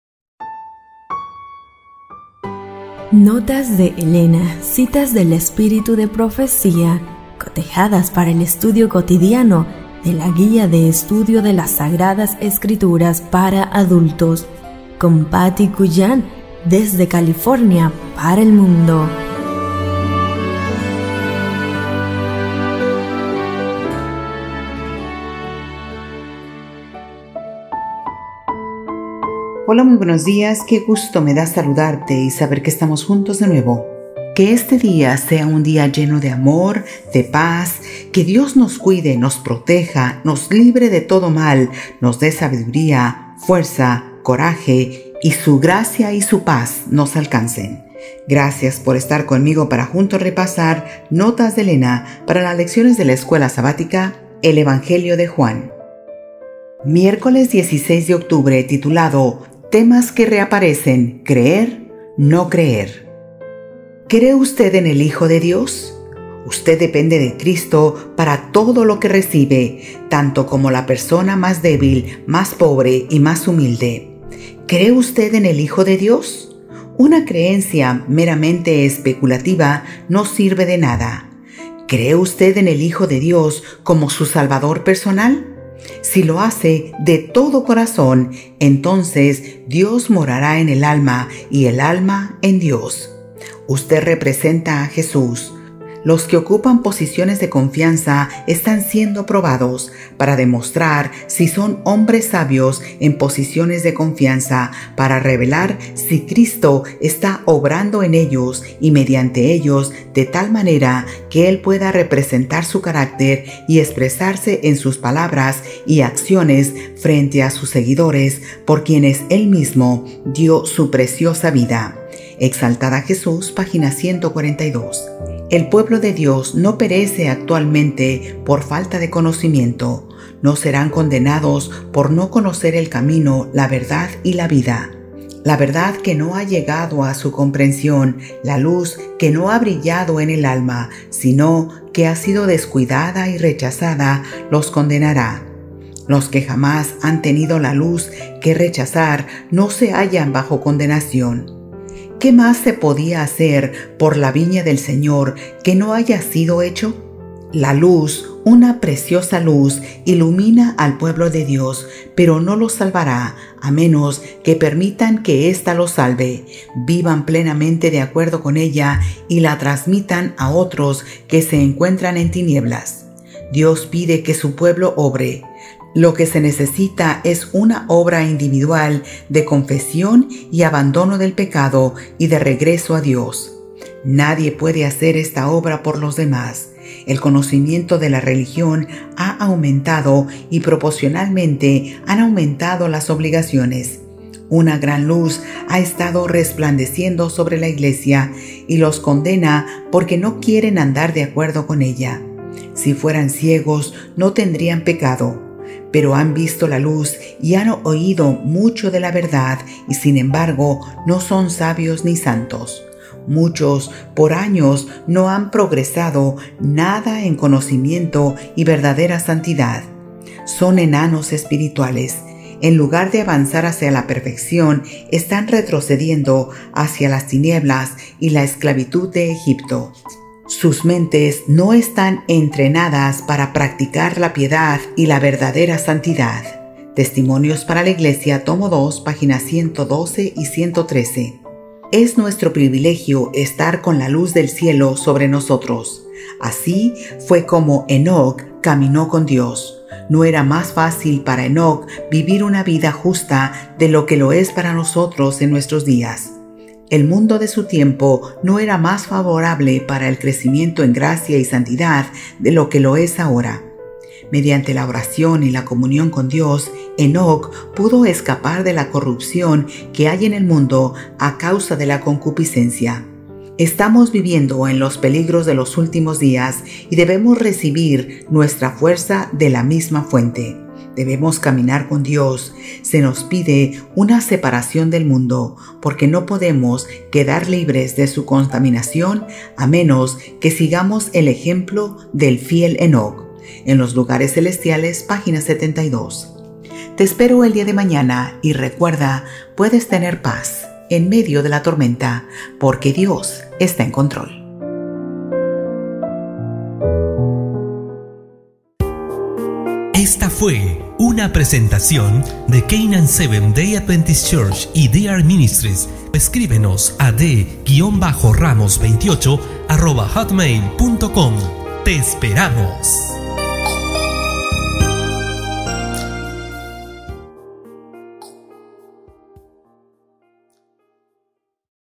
Notas de Elena - 16 de Octubre del 2024 - Temas que reaparecen: Creer/ no creer
MIÉRCOLES, 16 DE OCTUBRE
TEMAS QUE REAPARECEN: CREER/NO CREER
"¿Cree usted en el Hijo de Dios?" Usted depende de Cristo para todo lo que recibe, tanto como la persona más débil, más pobre y más humilde. "¿Cree usted en el Hijo de Dios?" Una creencia meramente especulativa no sirve de nada. ¿Cree usted en el Hijo de Dios como su Salvador personal? Si lo hace de todo corazón, entonces Dios mora en el alma, y el alma en Dios. Usted representa a Jesús. Los que ocupan posiciones de confianza están siendo probados, para demostrar si son hombres sabios en posiciones de confianza, para revelar si Cristo está obrando en ellos y mediante ellos de tal manera que él pueda representar su carácter y expresarse en sus palabras y acciones frente a sus seguidores, por quienes él mismo dio su preciosa vida (Exaltad a Jesús, p. 142).

El pueblo de Dios no perece actualmente por falta de conocimiento. No serán condenados por no conocer el camino, la verdad y la vida. La verdad que no ha llegado a su comprensión, la luz que no ha brillado en el alma, sino que ha sido descuidada y rechazada, los condenará. Los que jamás han tenido la luz que rechazar, no se hallan bajo condenación. ¿Qué más se podía hacer por la viña del Señor que no haya sido hecho? La luz, una preciosa luz, ilumina al pueblo de Dios; pero no los salvará, a menos que permitan que esta los salve, vivan plenamente de acuerdo con ella y la transmitan a otros que se encuentran en tinieblas. Dios pide que su pueblo obre. Lo que se necesita es una obra individual de confesión y abandono del pecado, y de regreso a Dios. Nadie puede hacer esta obra por los demás. El conocimiento de la religión ha aumentado, y proporcionalmente han aumentado las obligaciones. Una gran luz ha estado resplandeciendo sobre la iglesia, y los condena porque no quieren andar de acuerdo con ella. Si fueran ciegos, no tendrían pecado. Pero han visto la luz y han oído mucho de la verdad, y sin embargo no son sabios ni santos. Muchos, por años no han progresado nada en conocimiento y verdadera santidad. Son enanos espirituales. En lugar de avanzar hacia la perfección, están retrocediendo hacia zas tinieblas y la esclavitud de Egipto. Sus mentes no están entrenadas para practicar la piedad y la verdadera santidad (Testimonios para la iglesia, t. 2, pp. 112, 113).

Es nuestro privilegio estar con la luz del Cielo sobre nosotros. Así fue como Enoc caminó con Dios. No era más fácil para Enoc vivir una vida justa de lo que lo es para nosotros en nuestros días. El mundo de su tiempo no era más favorable para el crecimiento en gracia y santidad de lo que lo es ahora.

Mediante la oración y la comunión con Dios Enoc pudo escapar de la corrupción que hay en el mundo a causa de la concupiscencia. Estamos viviendo en los peligros de los últimos días, y debemos recibir nuestra fuerza de la misma Fuente. Debemos caminar con Dios. Se nos pide una separación del mundo, porque no podemos quedar libres de su contaminación a menos que sigamos el ejemplo del fiel Enoc (En los lugares celestiales, p. 72).

NOTAS DE ELENA
MATERIAL COMPLEMENTARIO DE ESCUELA SABÁTICA